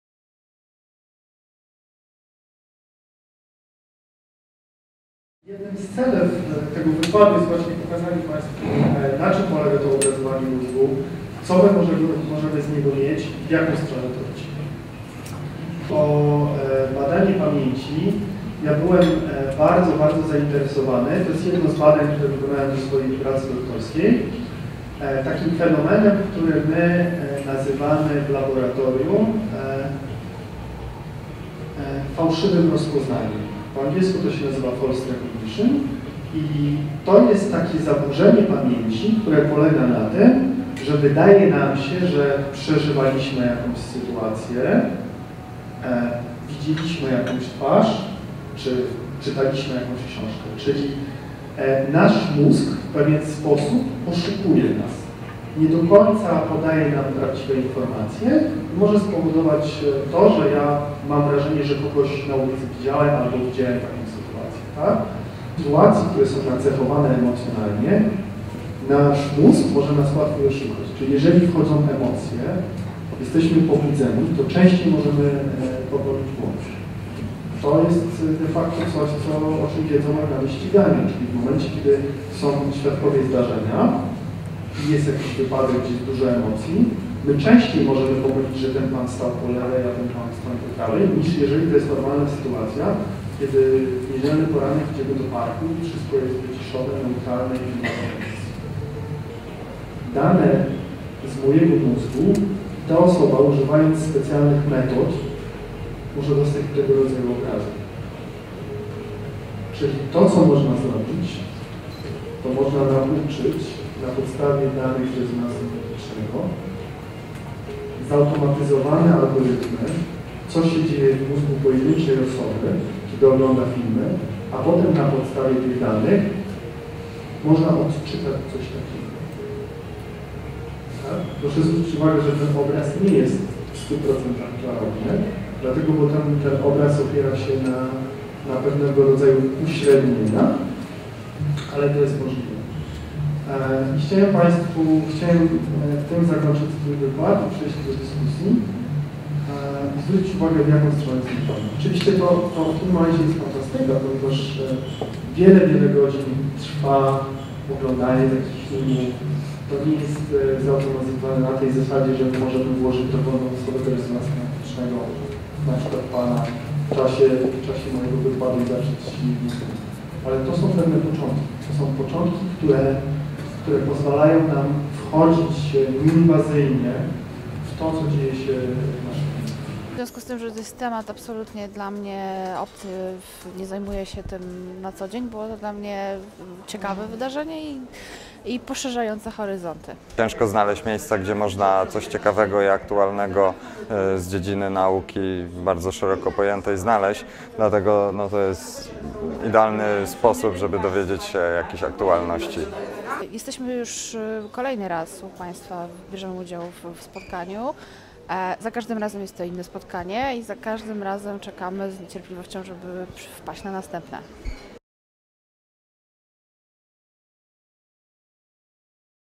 Relacja: Kłamstwo, płeć i obrazowanie mózgu -dr A. Marchewka 15.04.2013
Tytuł: Kłamstwo, płeć i obrazowanie mózgu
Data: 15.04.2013
Ekspert: Dr Artur Marchewka
Pracownia Obrazowania Mózgu, Centrum Neurobiologii, Instytut Biologii Doświadczalnej im. M. Nenckiego, Polska Akademia Nauk
Organizator: Rada Upowszechniania Nauki Polskiej Akademii Nauk, Festiwal Nauki Uniwersytetu Warszawskiego
Miejsce: Skwer Hoovera, Krakowskie Przedmieście 60a, Warszawa
Opis: Zaawansowane technologie radykalnie zmieniają sposób w jaki bada się działanie ludzkiego mózgu. Można prowadzić badania w trakcie wykonywania skomplikowanych operacji umysłowych. Możliwe jest odkodowywanie działania mózgu.

Prelegent prowadzi badania dotyczące między innymi procesów pamięci oraz kłamstwa. Przedstawił w skrócie najnowsze metody obrazowania mózgu z zastosowaniem rezonansu magnetycznego (MRI, fMRI). Przedstawił też kierunki w jakich zmierzają badania z wykorzystaniem metod neuroobrazowania.